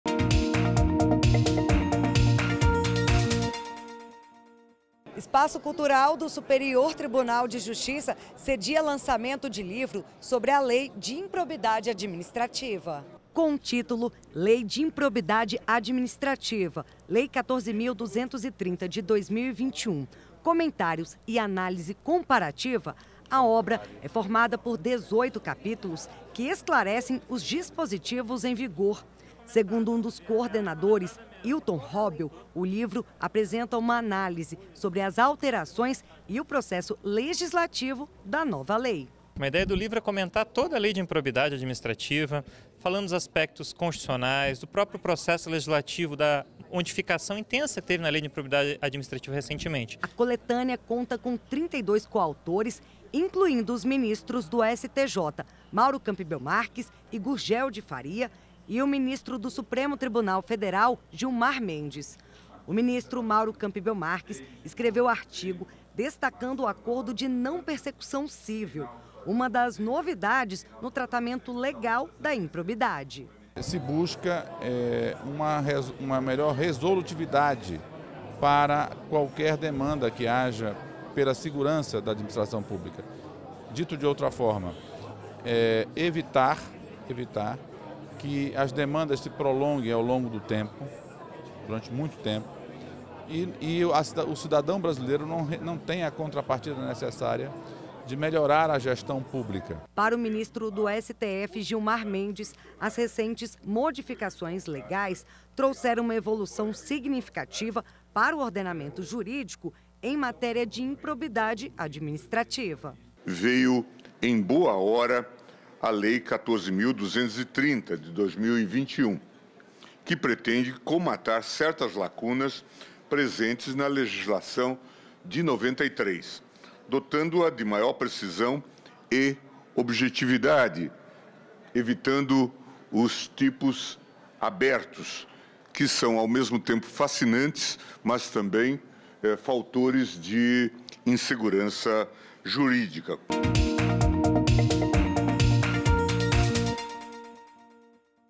Obra lançada no STJ discute as novidades no tratamento legal da improbidade administrativa
O Espaço Cultural STJ realizou, nesta terça-feira (20), o lançamento do livro Lei de Improbidade Administrativa (Lei 14.230/2021): comentários e análise comparativa. O evento aconteceu no mezanino do Edifício dos Plenários.
Coordenada por Wesley Rocha, membro titular do Conselho de Administração de Recursos Fiscais (CARF), e pelos professores Fábio Scopel Vanin e Ilton Norberto Robl Filho, a obra traz uma análise das alterações na Lei de Improbidade Administrativa, com reflexões sobre sua perspectiva constitucional e o processo legislativo. Com enfoque nas mudanças trazidas pela Lei 14.230/2021, o livro explica os dispositivos vigentes e seus impactos na administração pública brasileira.